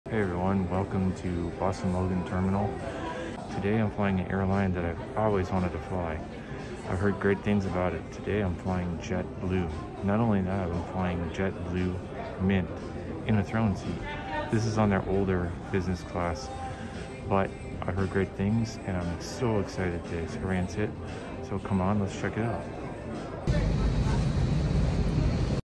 JetBlue Mint | See The Throne Seat at The End!
Intro Short talking about my JetBlue Mint Throne seat flight review!
See the throne seat at the end!